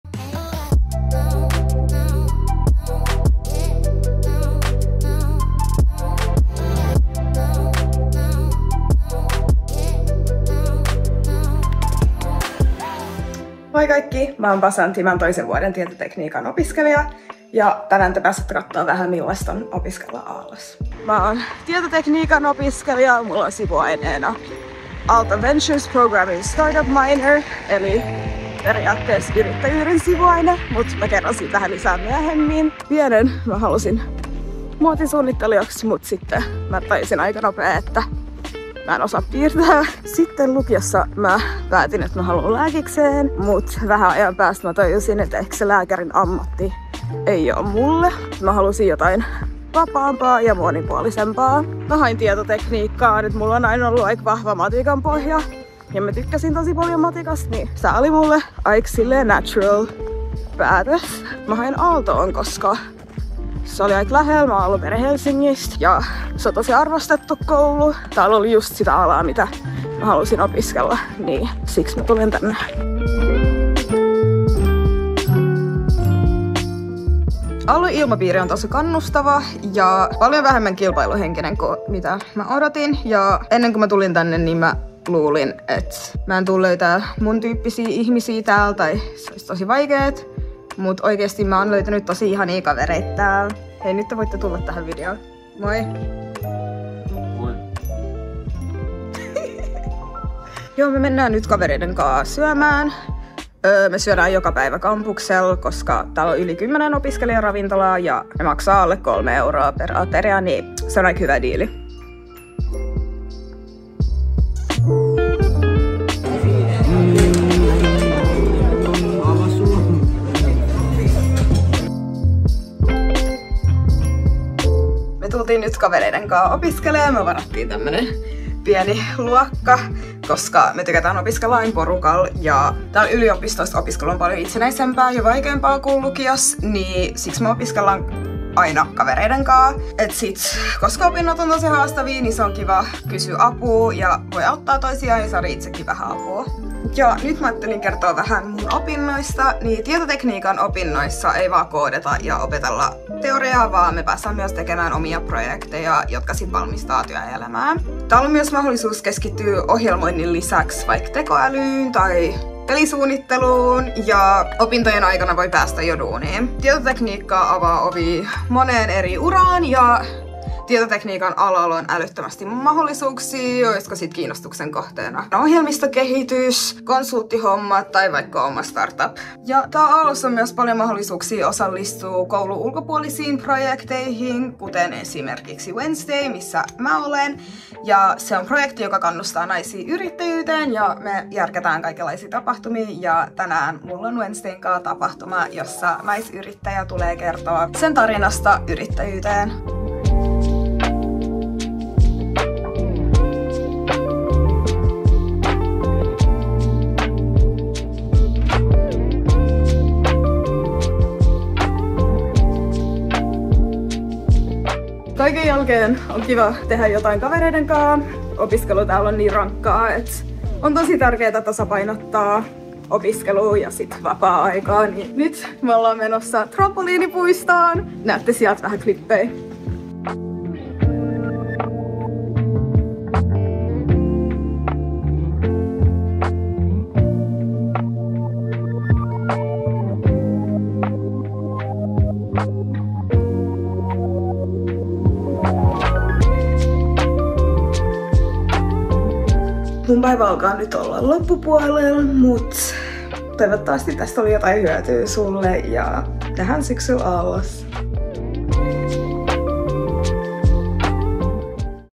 Tietotekniikka | Student vlog
Miltä näyttää tietotekniikan opiskelijan arki Aalto-yliopistossa ja mitä kaikkea opinnot sisältävät? Basant kertoo vlogissaan opiskelupäivästään ja opinnoistaan.

Tietotekniikan opinnot ovat kiehtova yhdistelmä insinööritieteitä, luonnontieteitä, ihmistieteitä ja jopa taidetta, sillä esimerkiksi moni tietokonepeli on taideteos siinä missä elokuvakin. Tutkinnosta voi tehdä hyvinkin omannäköisensä esimerkiksi sivuaineen ja vapaasti valittavien opintojen kautta. Alalle hakeutuu monenlaisia ihmisiä: tietokoneisiin keskittyviä ammattilaisia, luovia taiteilijasieluja ja laaja-alaisia, ihmisten tarpeista, tiimityöstä ja liiketoiminnasta kiinnostuneita tekijöitä.